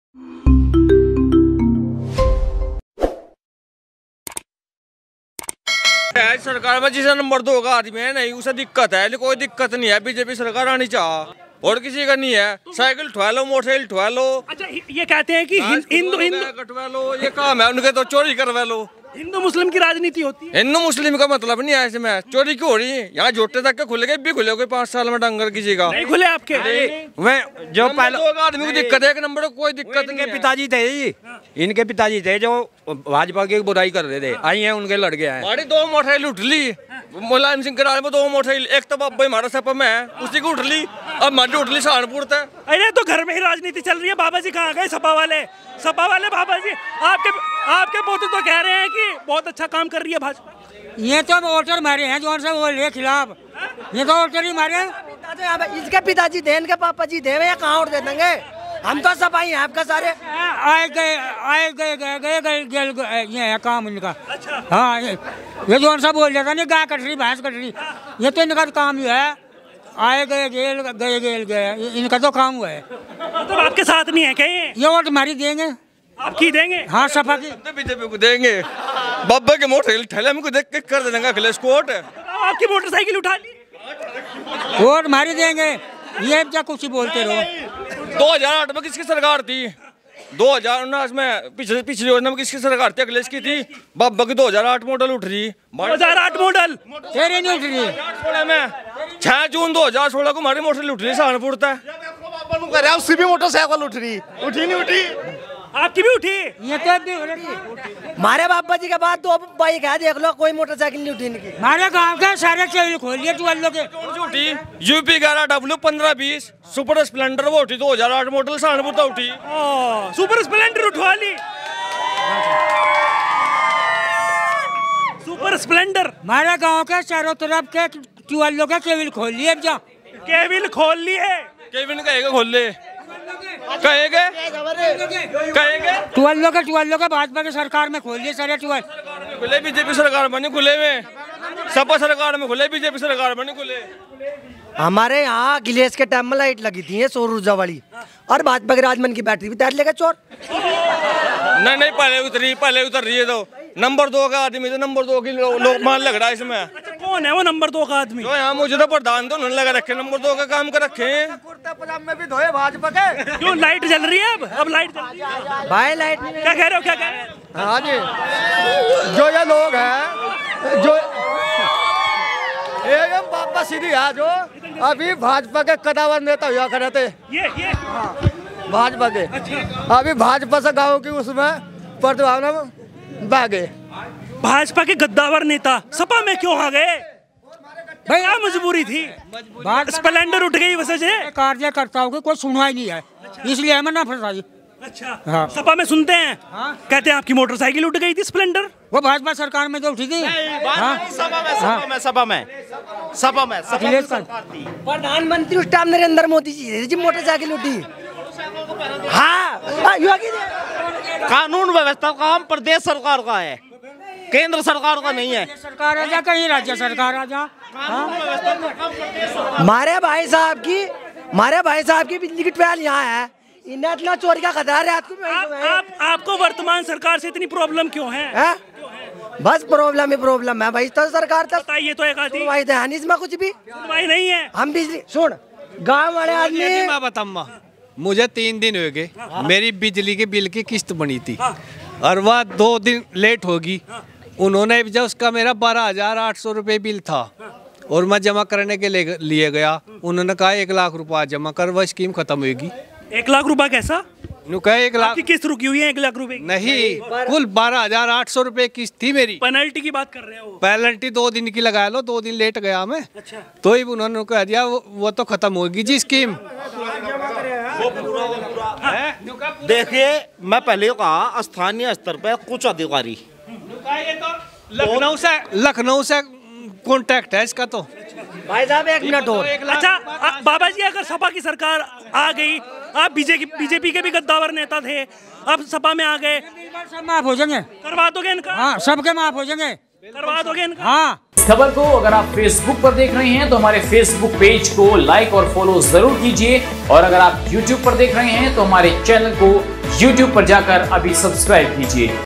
नानौता के खुडाना गांव में योगी अखिलेश को लेकर क्यों भिड़ पड़े बाबा और पोते !
नानौता के खुडाना गांव में योगी अखिलेश को लेकर क्यों भिड़ पड़े बाबा और पोते ! Watch Out Full Video And News !